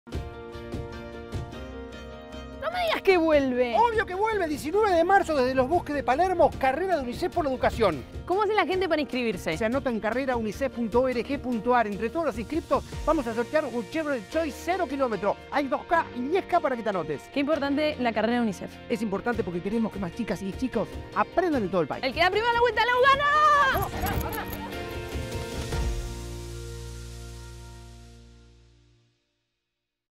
CarreraUNICEF por la Educación – 19 de marzo 2023
Vuelve la Carrera UNICEF por la Educación. Sumate para que más chicas y chicos aprendan en todo el país.

Toda la información la encontrás en carreraunicef.org.ar ¡Inscribite ahora!